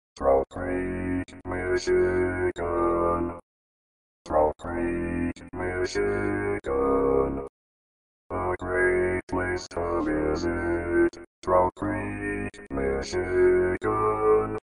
How to Say or Pronounce USA Cities — Trout Creek, Michigan
This video shows you how to say or pronounce Trout Creek, Michigan.

A computer said Trout Creek, Michigan. How would you say Trout Creek, Michigan?